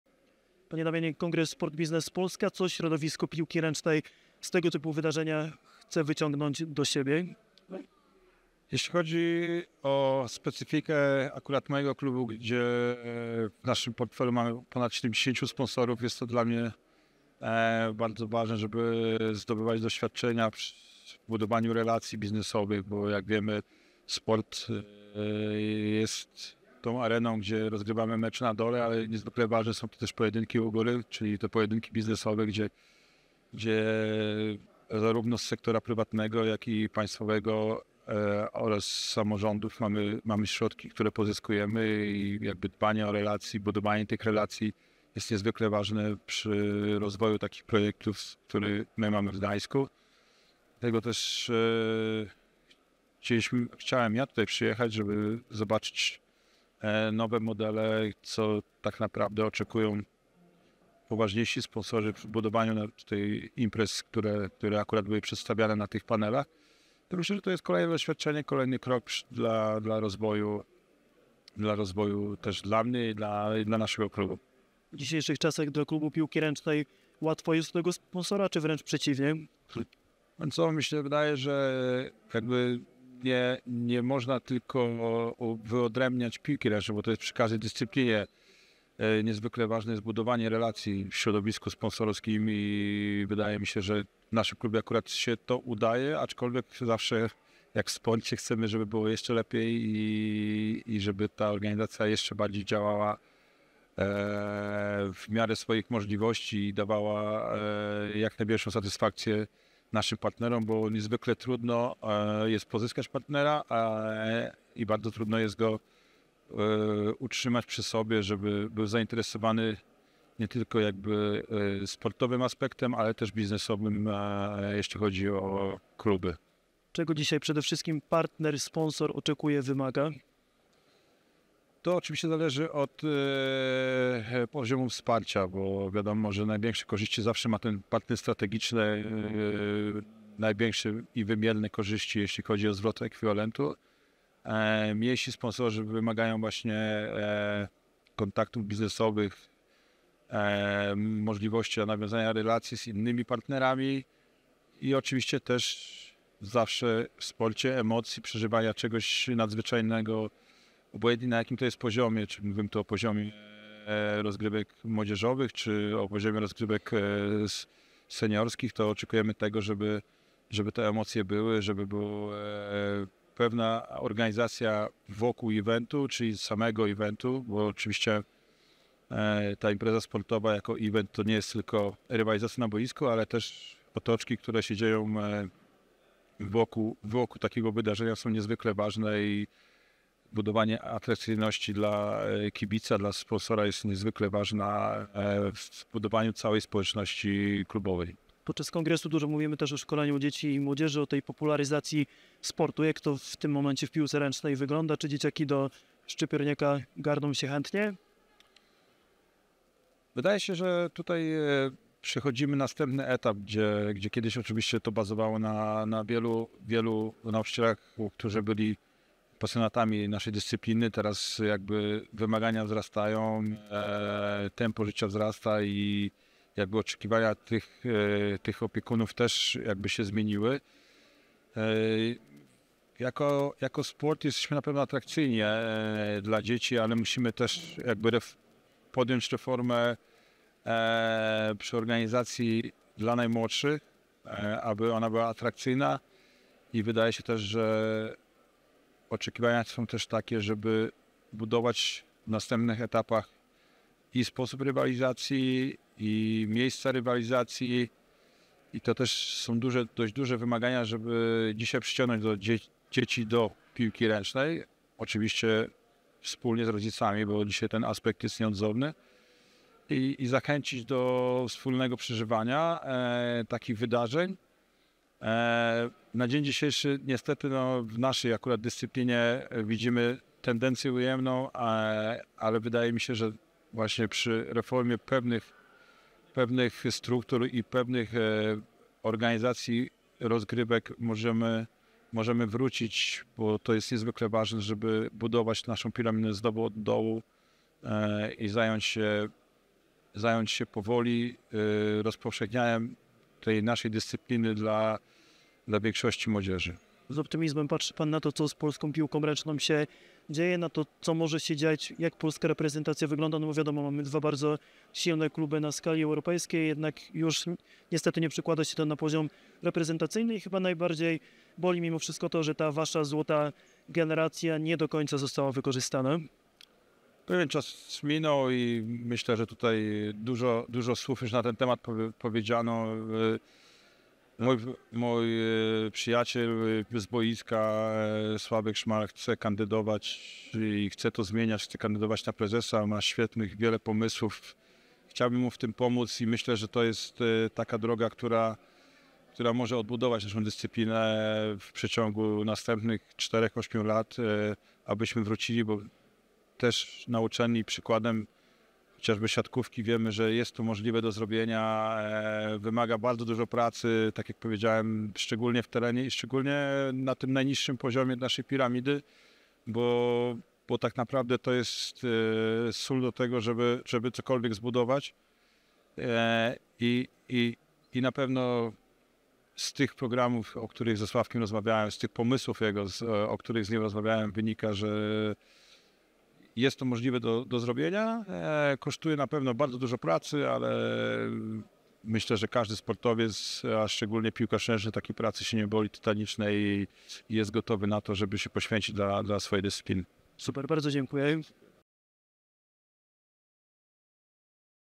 Damian Wleklak: KLUCZOWE JEST BUDOWANIE ATRAKCYJNOŚCI I RELACJI | SportMarketing.pl | SBP2024
Podczas VII Kongresu Sport Biznes Polska porozmawialiśmy z Damianem Wleklakiem, byłym piłkarzem ręcznym a obecnie managerem w Energa Wybrzeżu Gdańsk. Głównymi tematami rozmowy było budowanie relacji partnerskich z podmiotami wspierającymi sport oraz rozwój polskiej piłki ręcznej - od poziomu juniorskiego, aż do reprezentacji.

Partnerem merytorycznym materiału jest Stowarzyszenie Sport Biznes Polska.